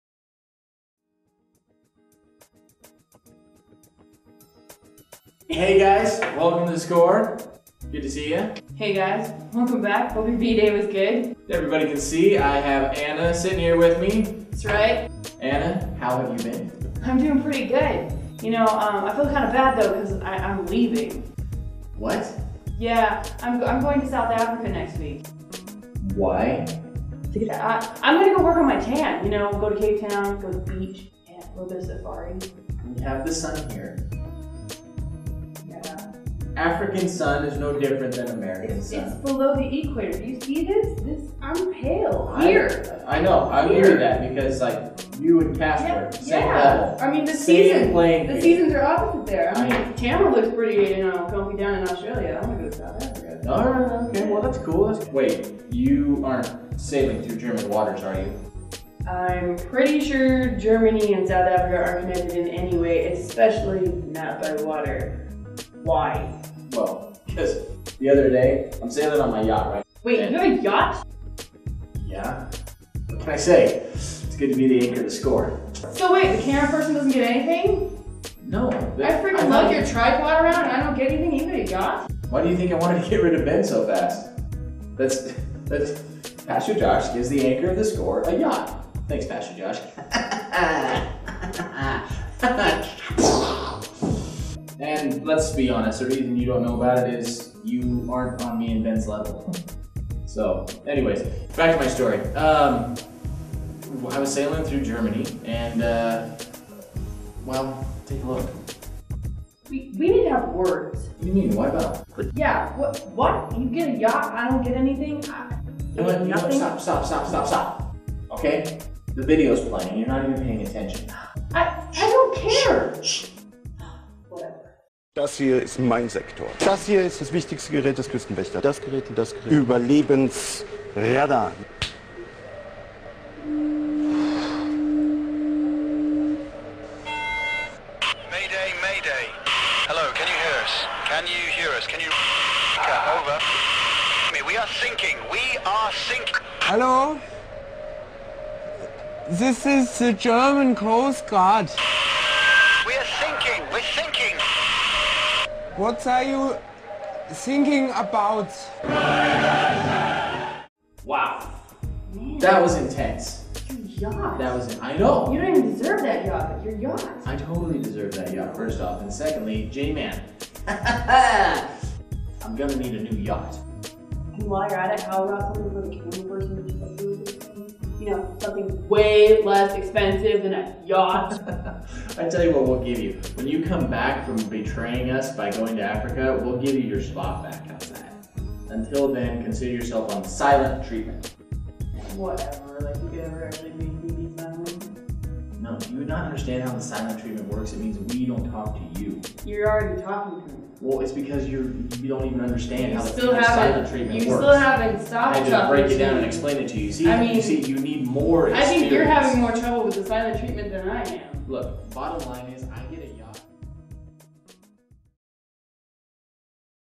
German Coast Guard Score
Matt says goodbye to Anna as she leaves for South Africa. Matt learns a geography lesson as he finds out Germany is no where near South Africa. Includes the German Coast Guard clip.